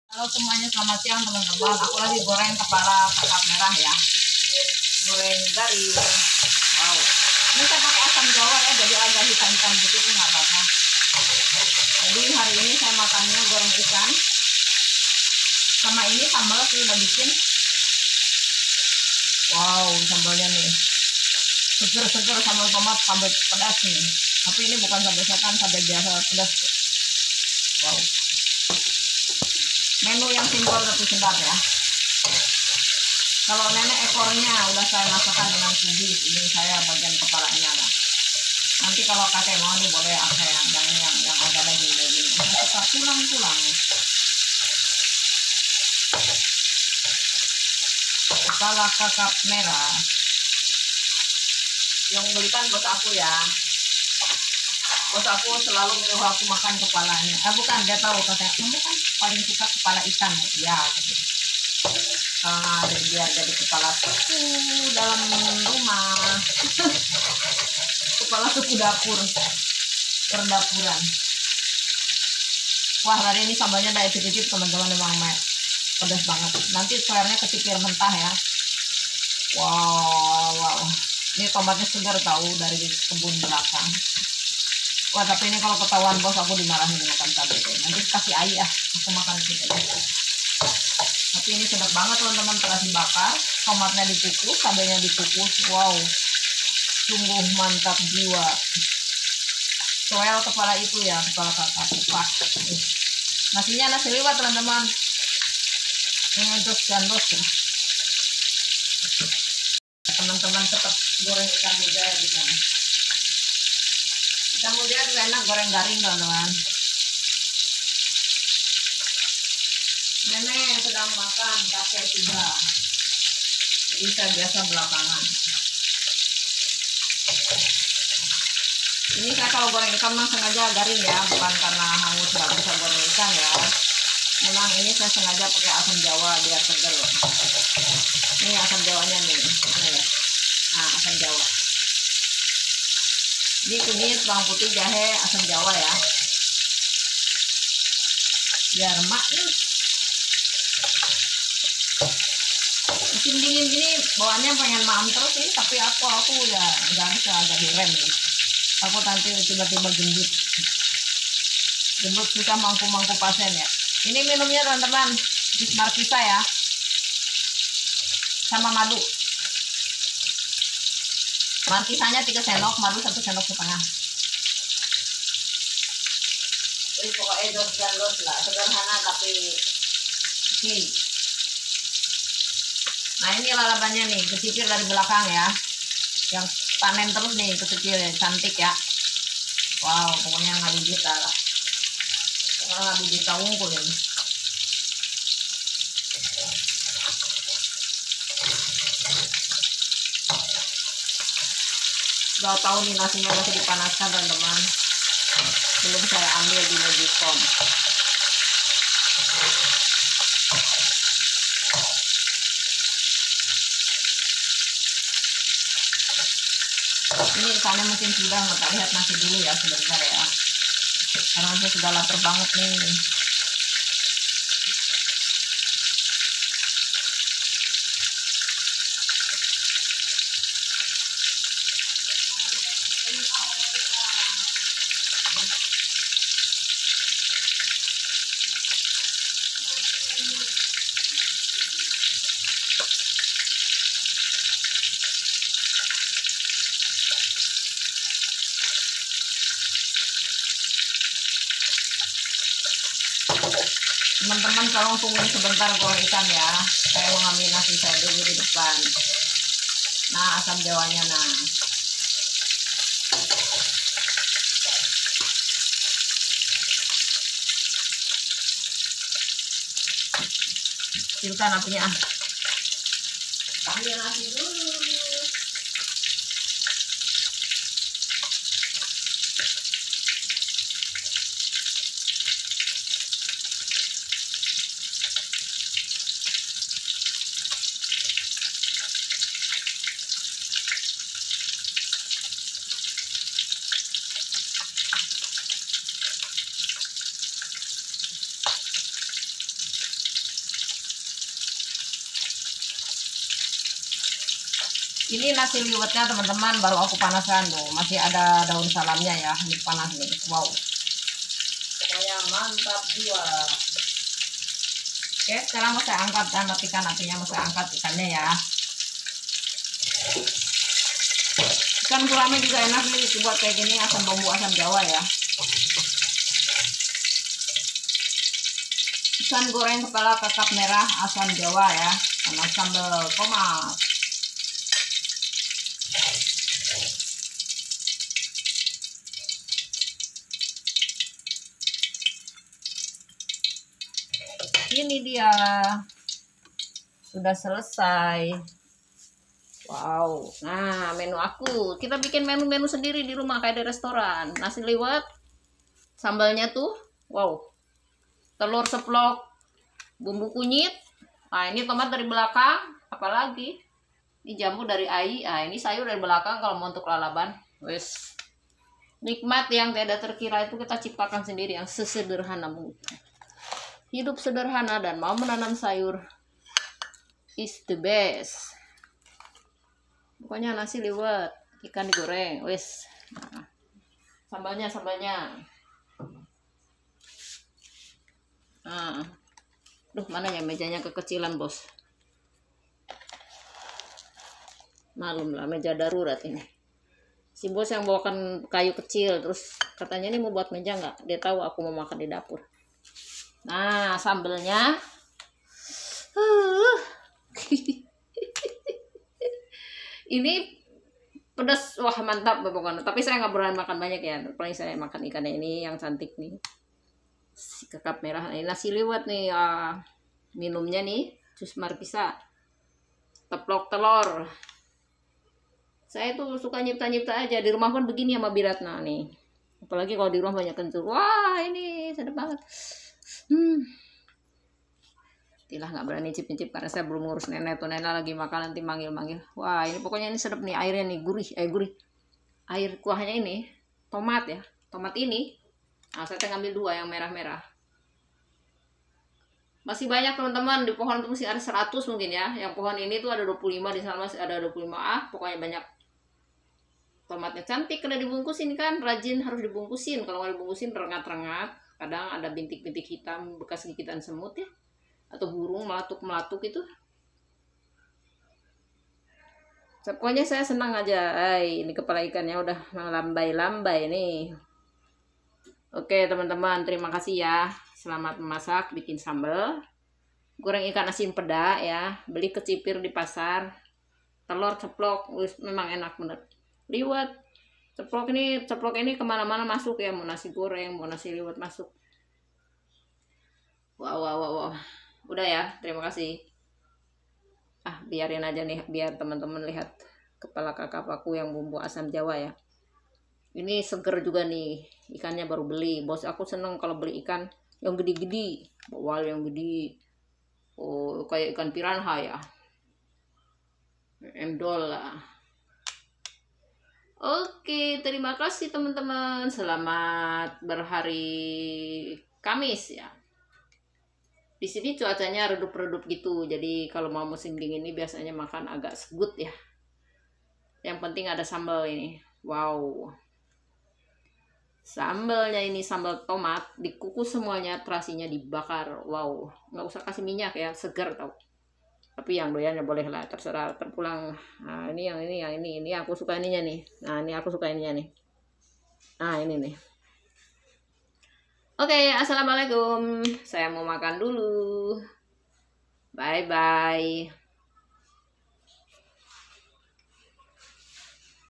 Goreng Kepala Kakap Sambel Pedas//Sedap Mantap//Rosmiati Salim//
Goreng Kepala Kakap Sambel Pedas//Rosmiati Salim//

Assalamualaikum Salam Sejahtera Untuk Kita Semuanya Yuk Masak Goreng Kepala Ikan Kakap Bumbui Dengan Asam Jawa Sebesar Ibu Jari Serbuk Bawang Putih Serbuk Kunyit Setgh Sdt Garam Penyedap Rasa Sedikit Merica Sedikit Serbuk Cili Diamkan 15 Menit Lalu Goreng Padukan Dengan Sambel Tomat Pedas Lalapan Selamat Memasak Jangan Lupa Subscribe Like Coment Juga Sharenya Ya Terimakasih,🙏